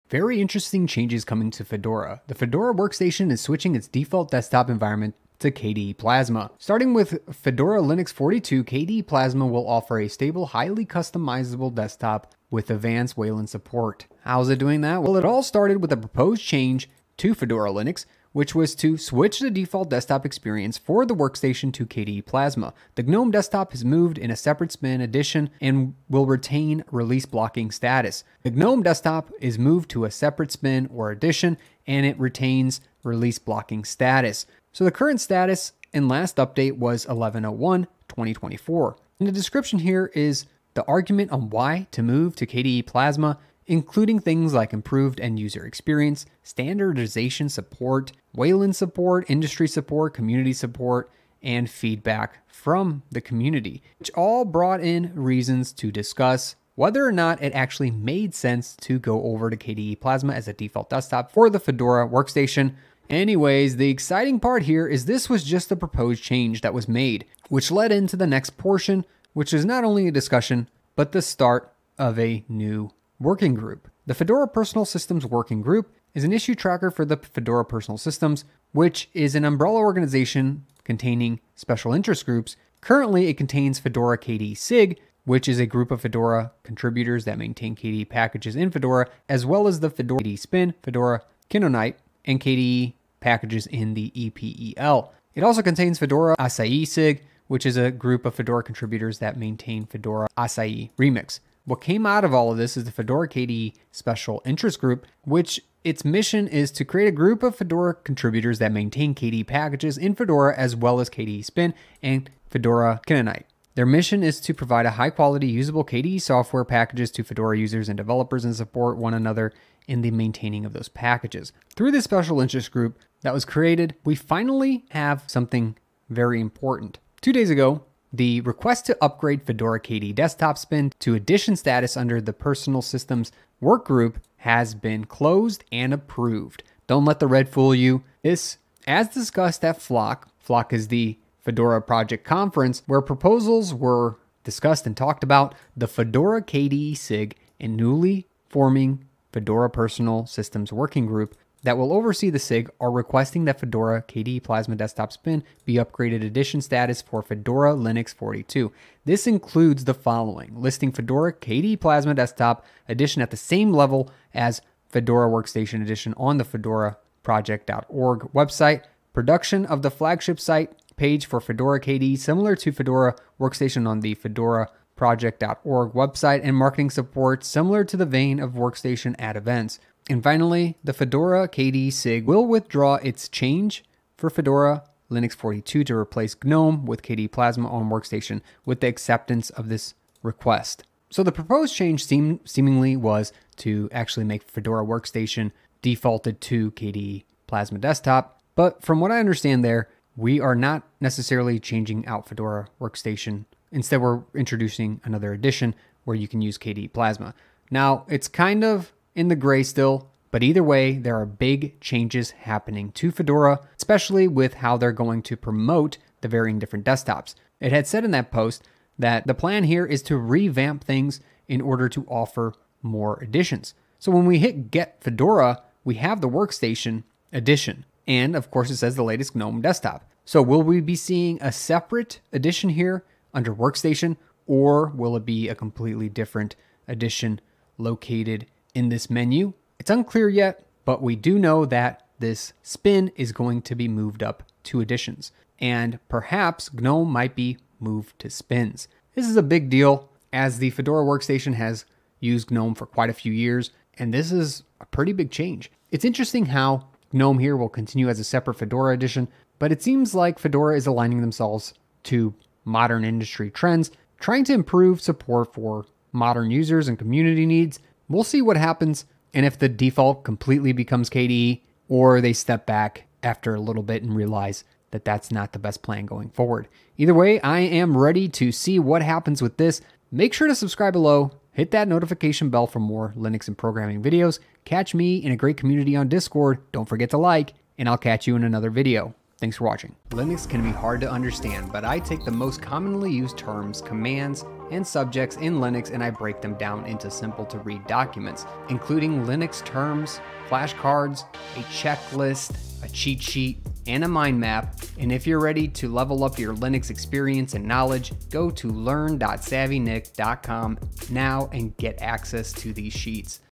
Fedora is Starting to Favor KDE Plasma.. A NEW SPIN?
Fedora is starting to look into switching desktop environments for its major workstation release. Fedora Linux is a very popular Linux distro so why would they be making this change?